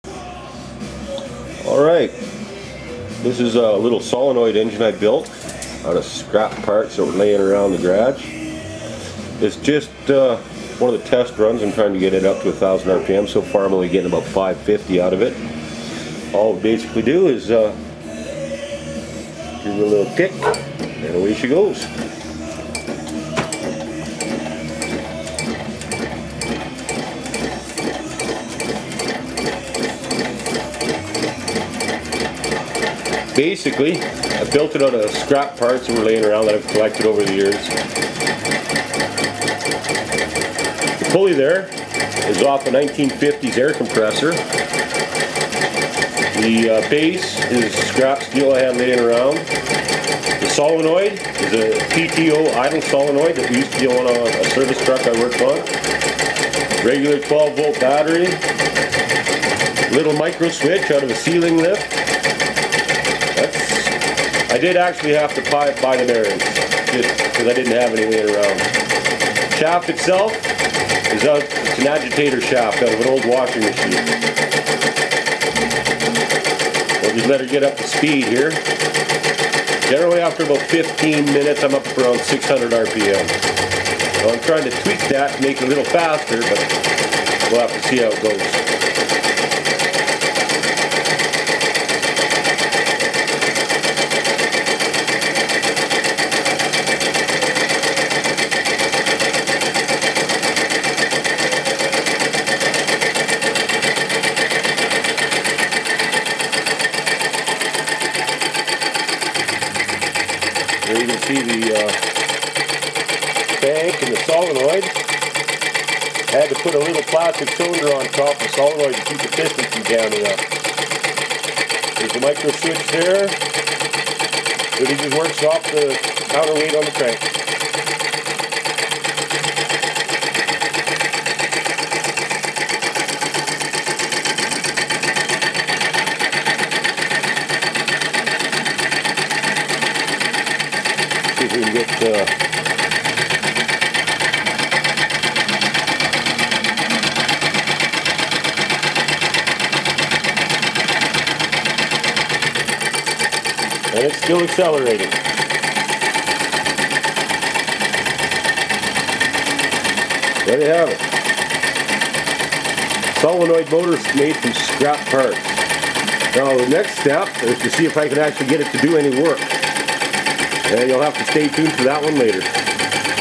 Solenoid Engine 1
I'm stalled on the dozer for a bit, til I can afford more steel. so I built this.
Test running a solenoid engine built from scrap parts.
Flywheel is a pulley from an old air compressor, shaft is the agitator shaft from and old washing machine, solenoid is a PTO idle solenoid from an old service truck, and the microswitch is from a handicapped ceiling lift. Top speed so far is around 600 RPM, but I think tweaking the timing will help with that..